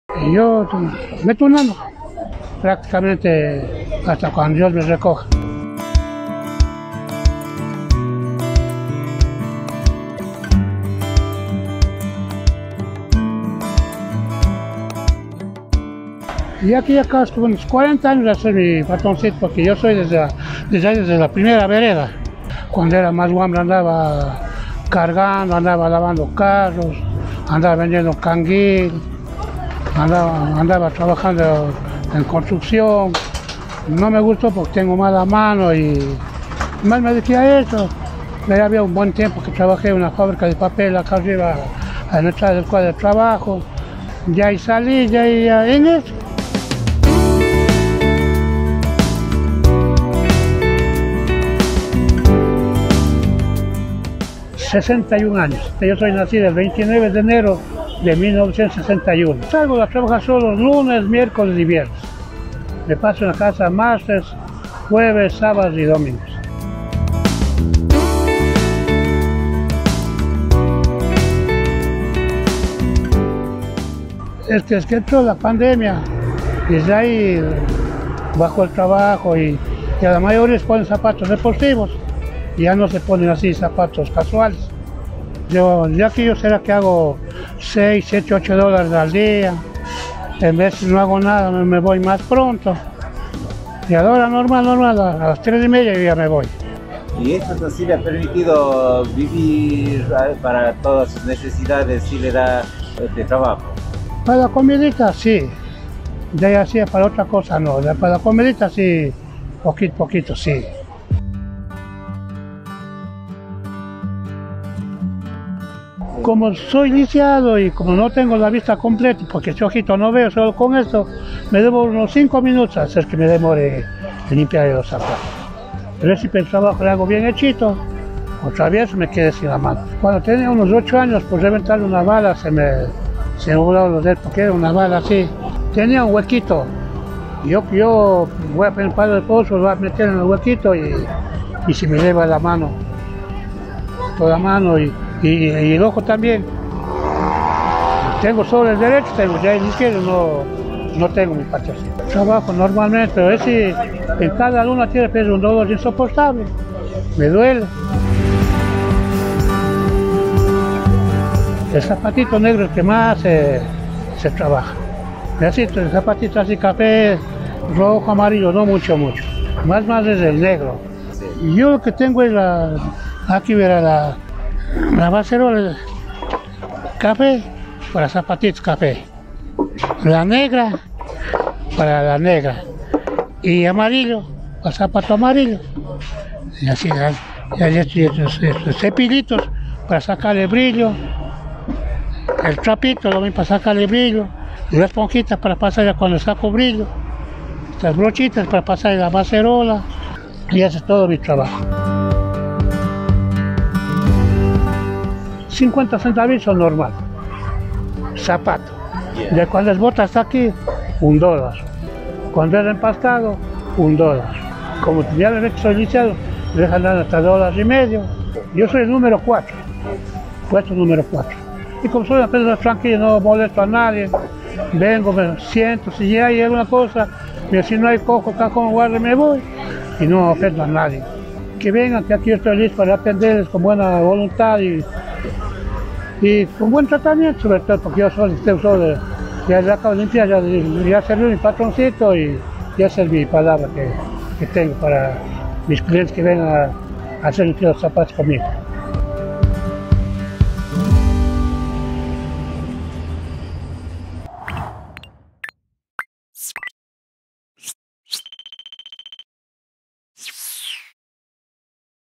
La mayor parte de la vida, Carlos Villa ha dedicado a betunear zapatos
El uso de calzado deportivo ha disminuido notablemente el trabajo de los betuneros o limpiabotas, un oficio que décadas atrás tenía mucha mayor presencia en todos los pueblos y, que en en la actualidad en ciertas localidades prácticamente ya no existe. Carlos Villa, quien sufrió en accidente cuando niño, perdió tres dedos y lleva 40 años dedicado a esta actividad que le permite subsistir.